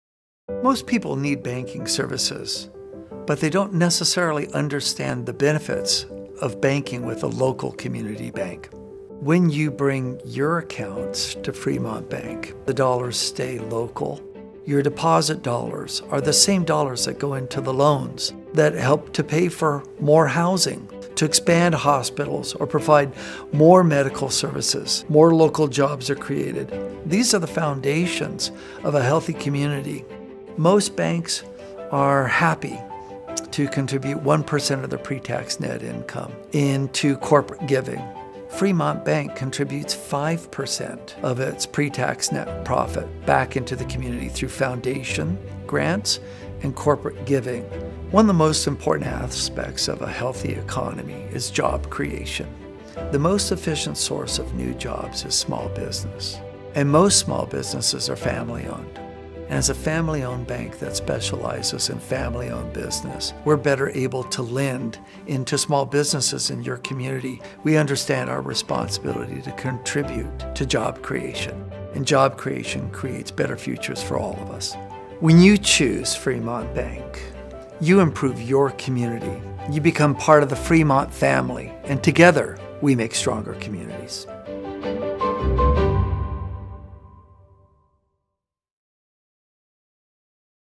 The value of community banking | Fremont Bank
Your deposit dollars are the same dollars that create loans for local business owners and homeowners, expand local services, and ultimately create more opportunity in your community.